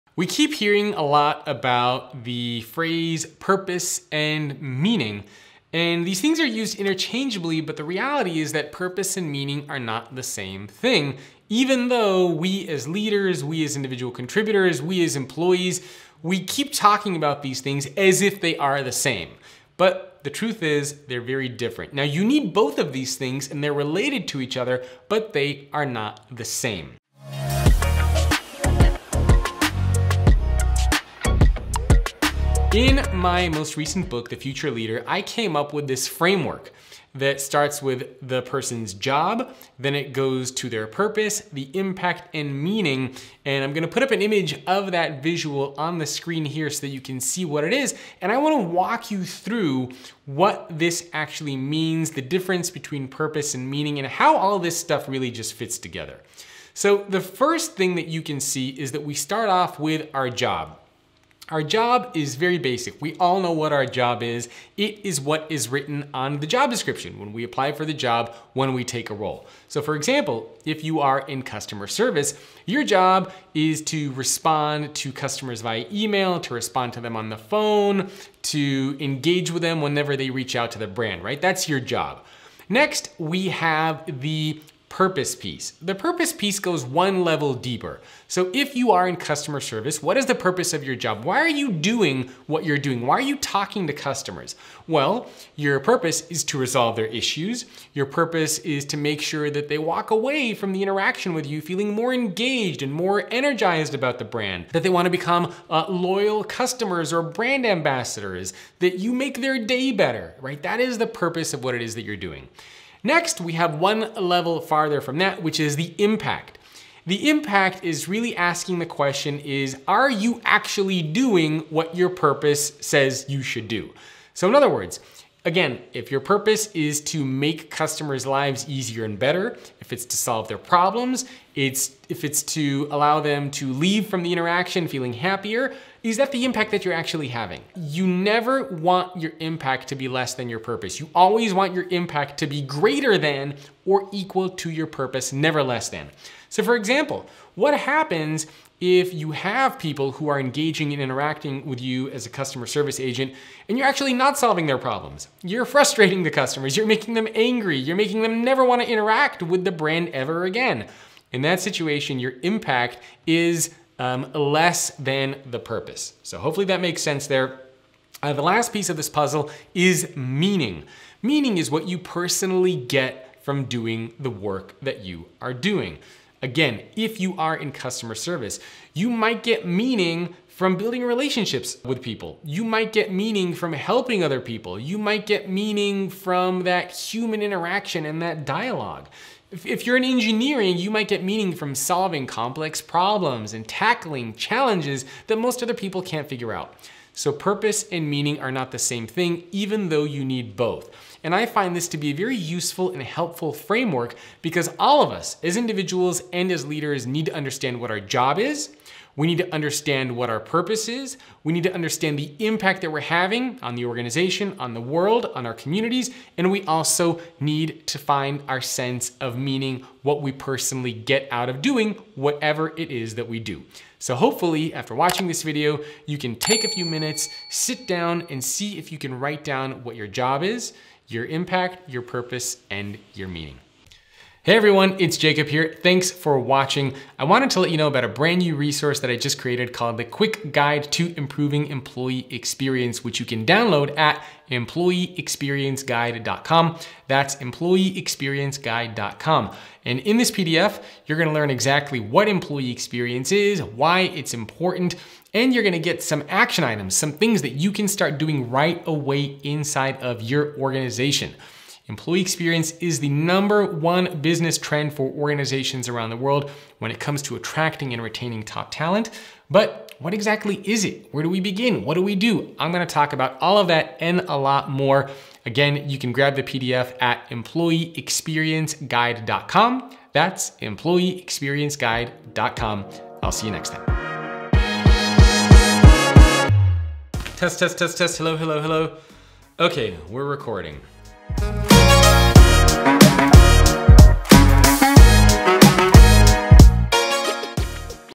The Difference Between Purpose and Meaning Plus How to Find Yours | Jacob Morgan
There are four parts to creating meaningful work: job, purpose, impact, and meaning.

Creating more purpose and meaning inside our organizations and inside our lives starts with understanding the difference between purpose and meaning.

We all deserve to have a sense of purpose and meaning from the jobs, the work, the organizations, and the leaders who we work with and for.

We all deserve to understand how the work we're doing impacts society and those around us.